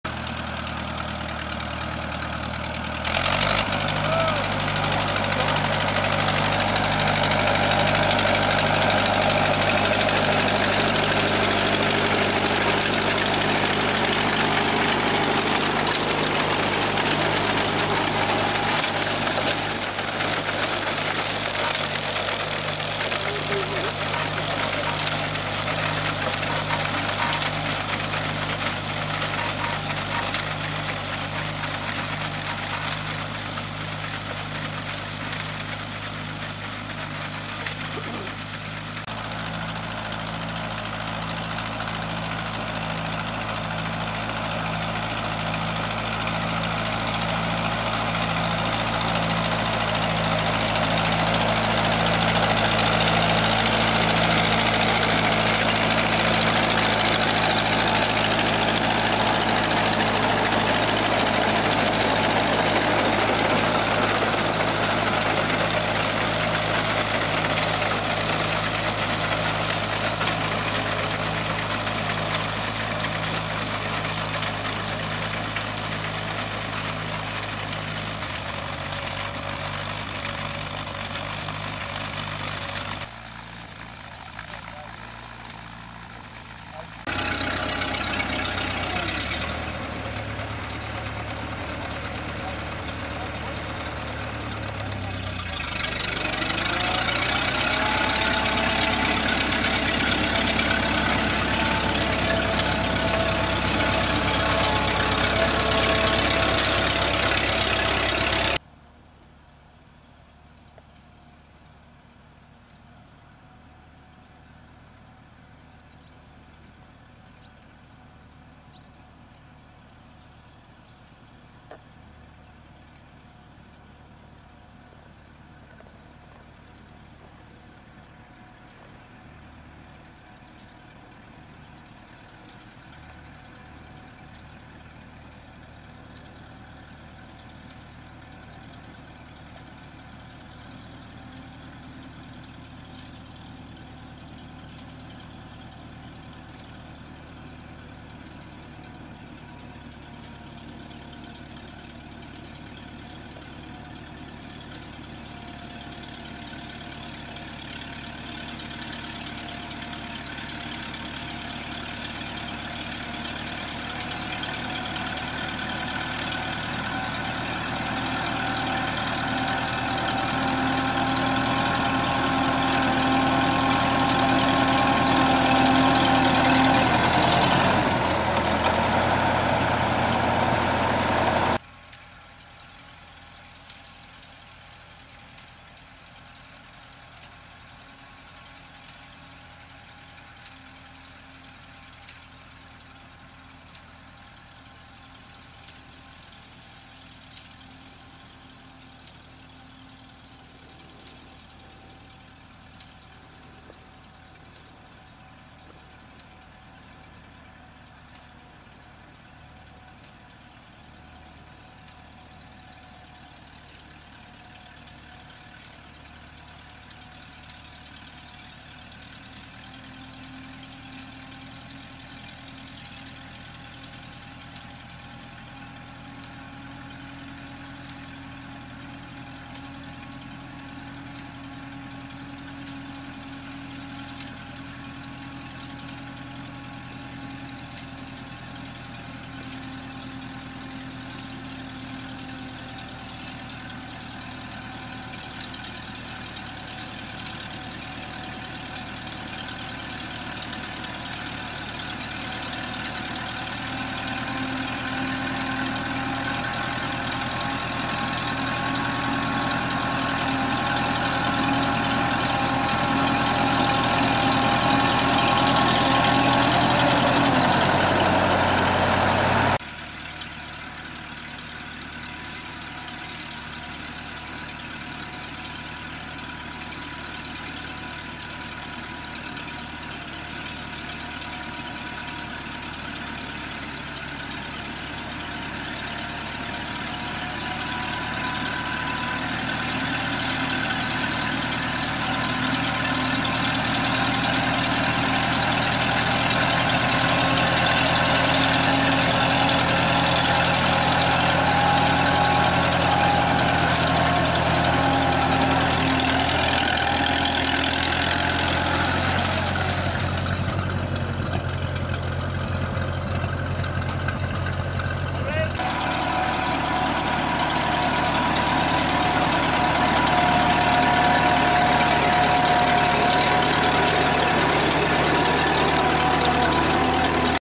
Harvesting the old fashioned way
Harvesting using a binder. Land kindly provided by Jim Morton, Kaeside Farm, Melrose. Produced: Norman Mitchinson